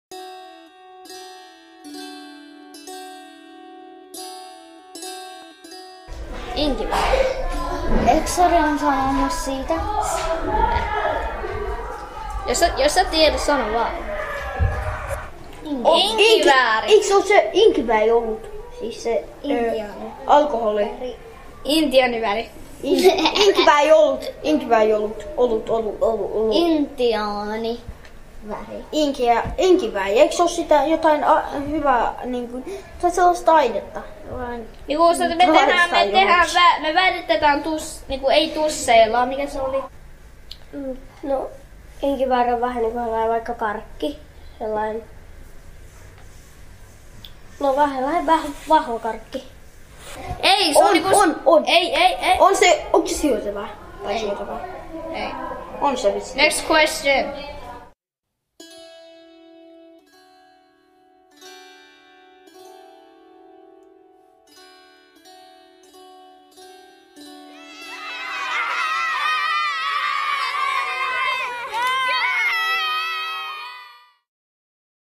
Luukku 21: Inkivääri
Inkivääri on trooppinen ruohokasvi. Siitä saatavaa maustetta käytetään joulun aikaan esimerkiksi piparkakkuihin ja jälkiruokiin.

Suomen murteissa inkivääri-lainasana on saanut monenlaisia muotoja: inkefääri, inkevääri, ink(i)fääri, inkivärri, inkiväärtti, inkvääri, inkväärni.